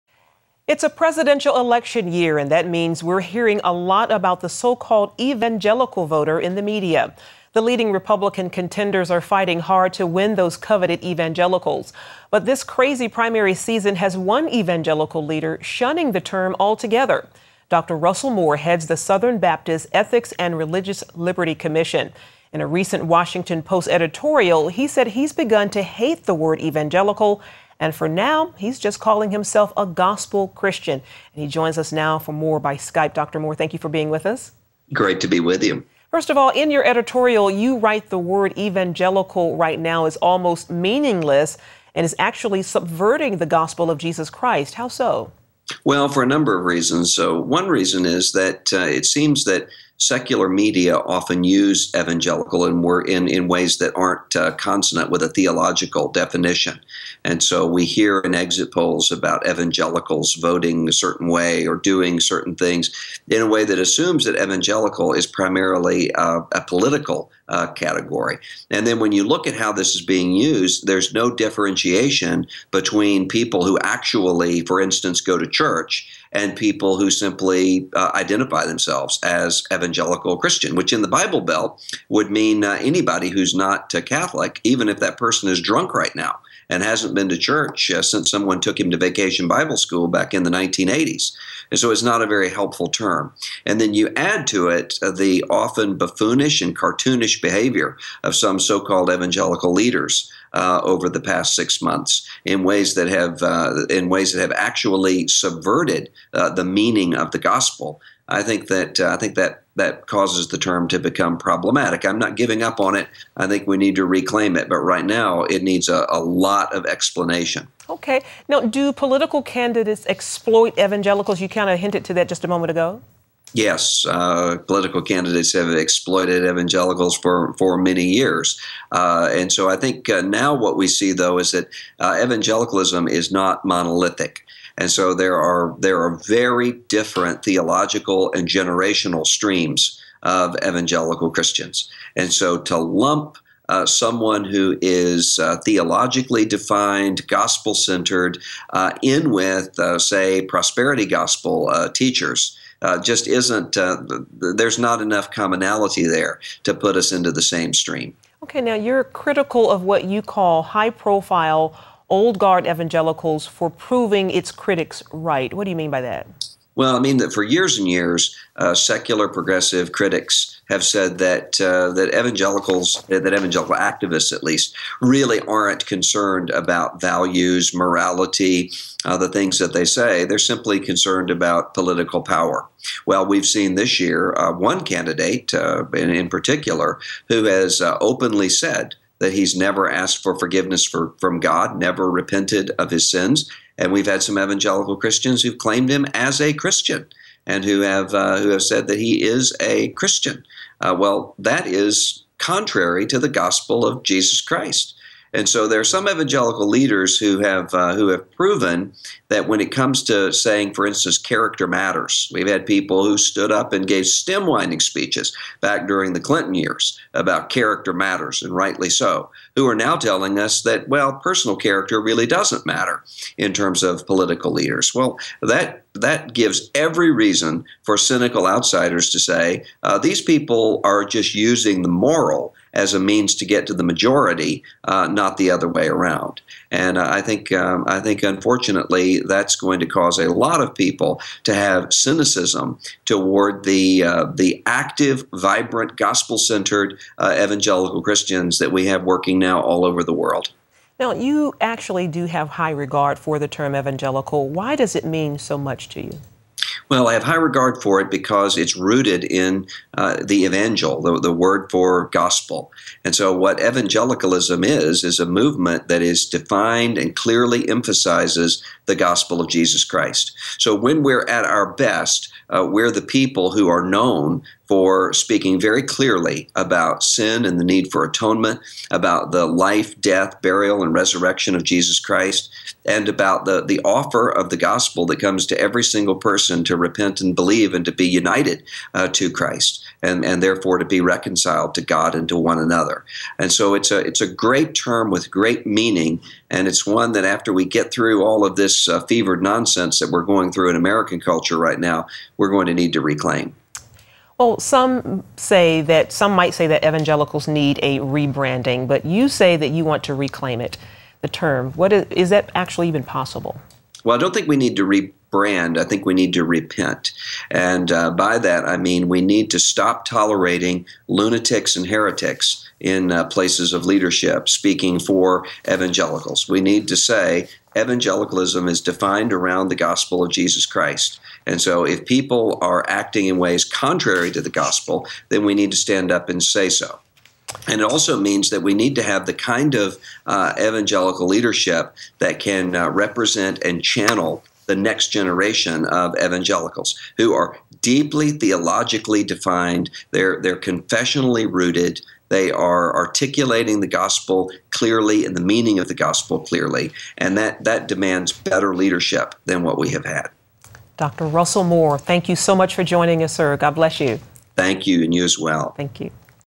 Russell Moore: "Don't Call Me Evangelical...For Now"
In a presidential election year there's a lot of talk in the media about the so-called "evangelical voter."